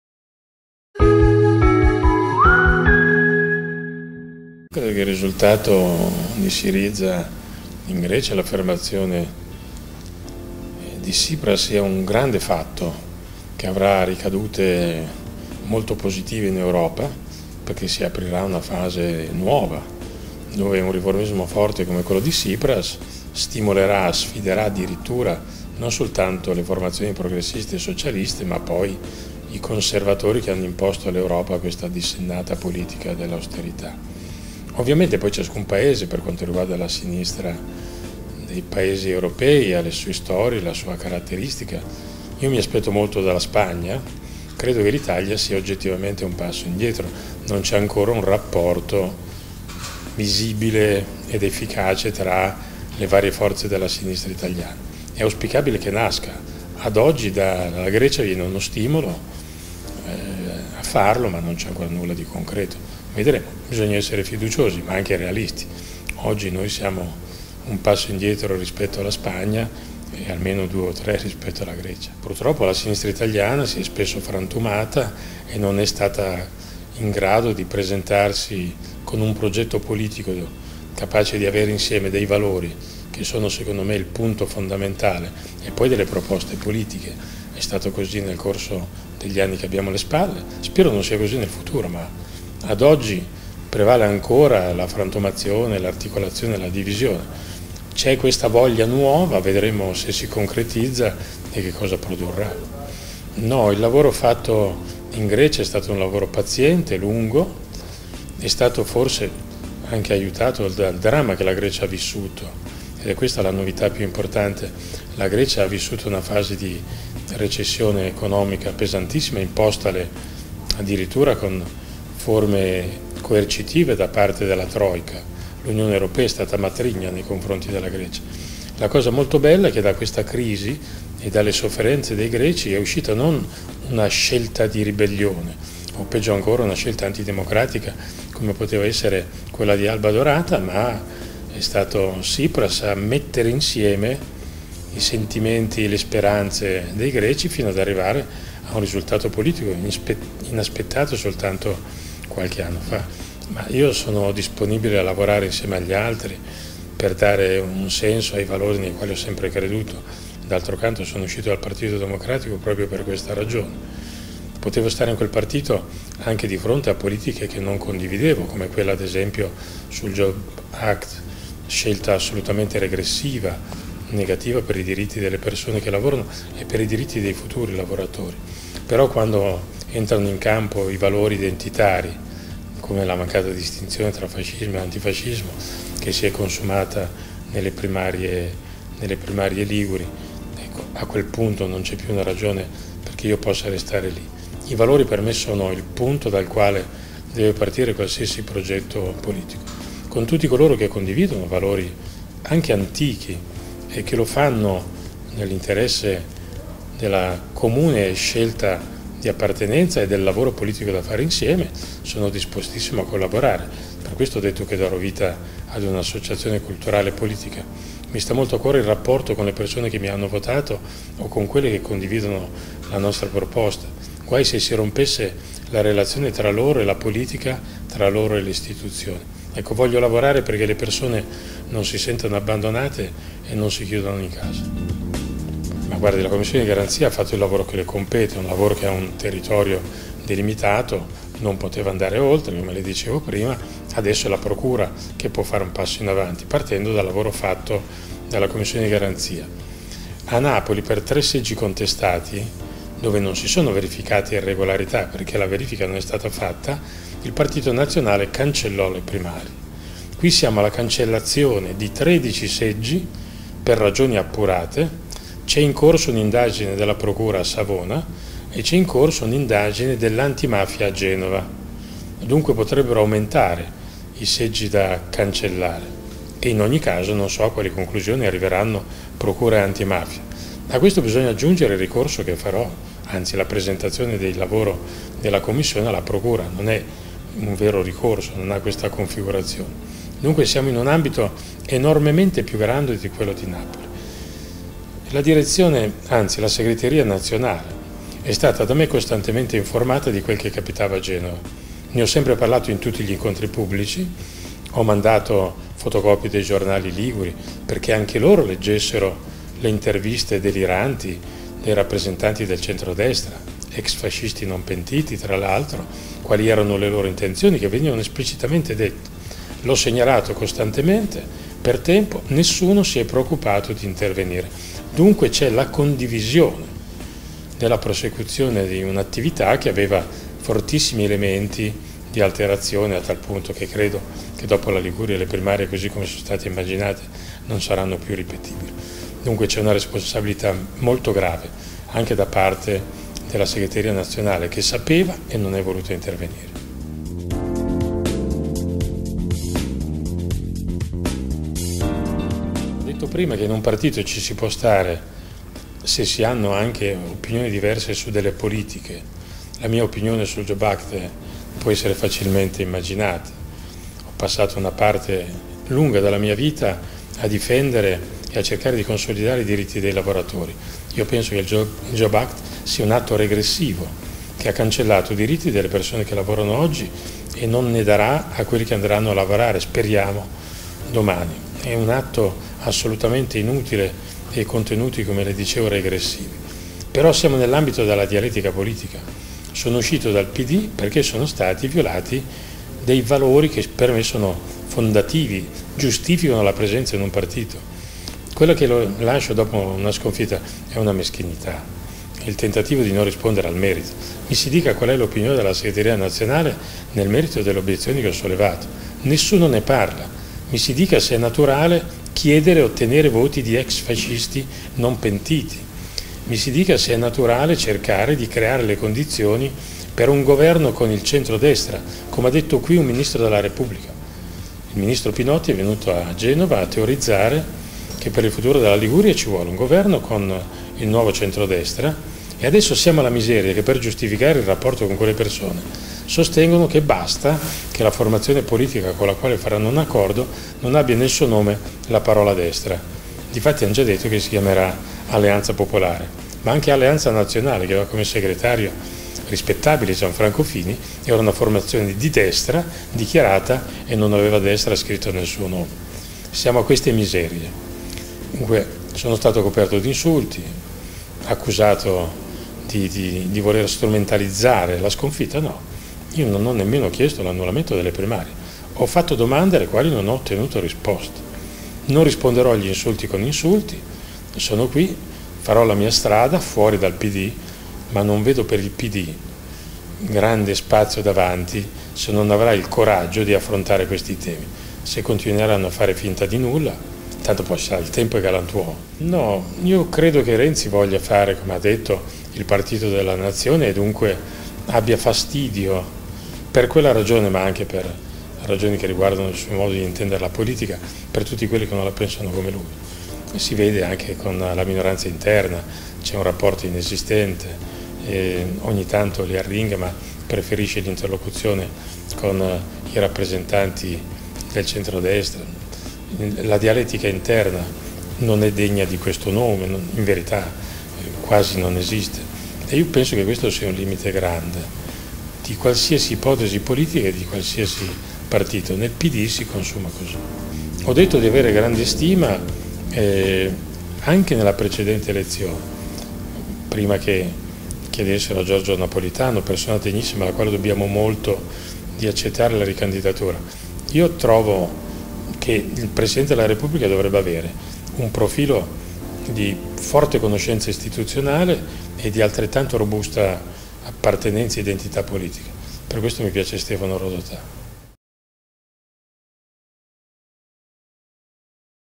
Cofferati: "Farò la mia strada fuori dal PD. Renzi non tollera chi non la pensa come lui"
Cofferati commenta il successo di Tsipras immaginando che ciò possa avere una ricaduta molto positiva in Europa, anche se "l'Italia è molto indietro rispetto alla Grecia".
Poi argomenta sull'attuale situazione della sinistra italiana e su chi pensa possa essere adatto al ruolo di prossimo Presidente della Repubblica.